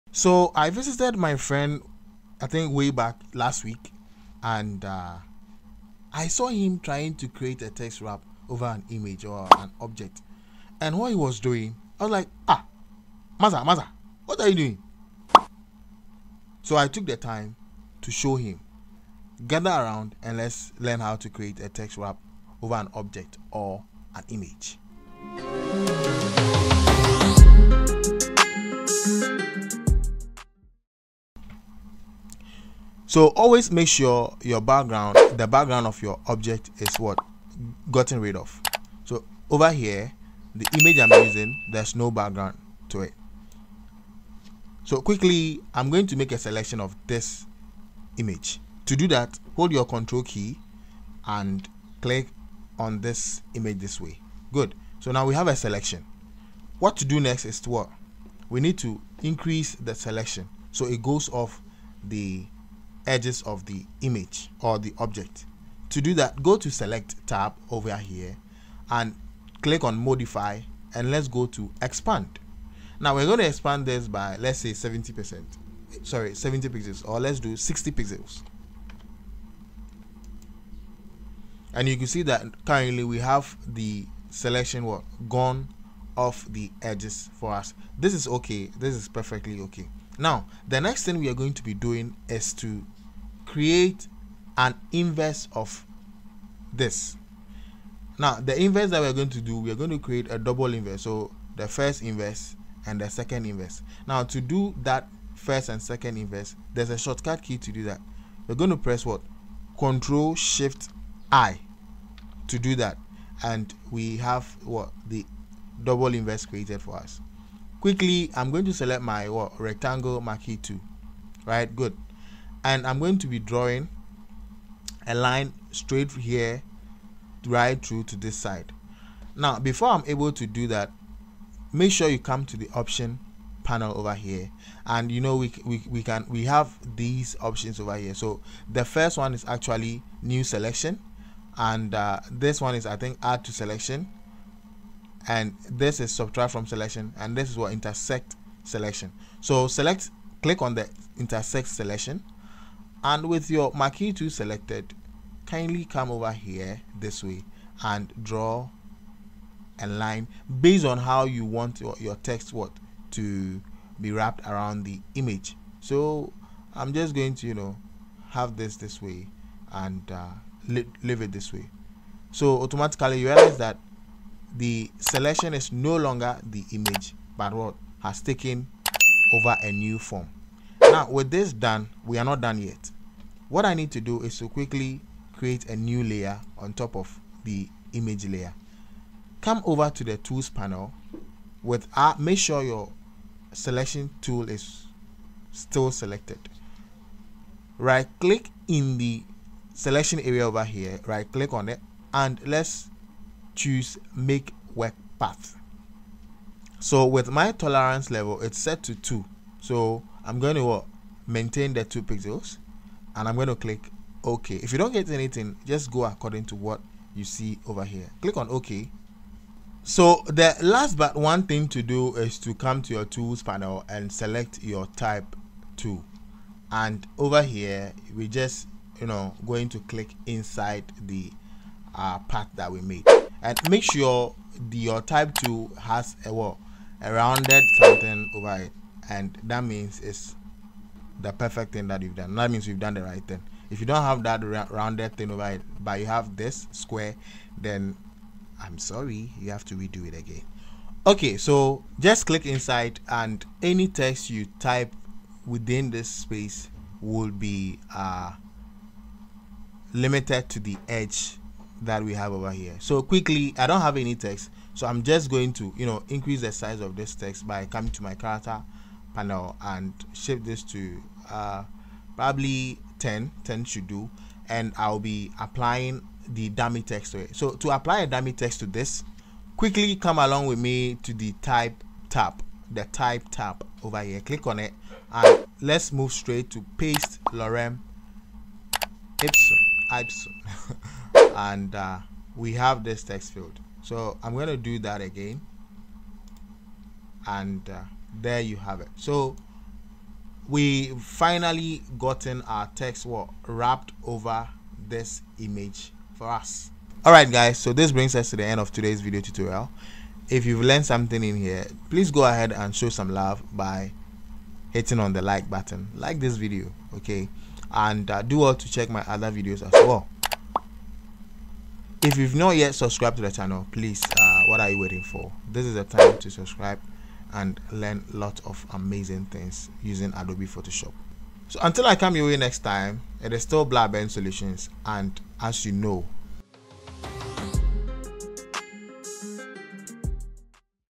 How to Wrap text around any image in Photoshop
Want to learn how to wrap text around any image in Photoshop like a pro? In this tutorial, I’ll show you a simple and effective way to make your text flow smoothly around any shape or object. Whether you're working on a flyer, social media design, or professional layout, this step-by-step guide will help you master text wrapping in Photoshop in just a few minutes!

I also take orders for Designing, Printing, and Transcription Services

With regards,
Team Blaben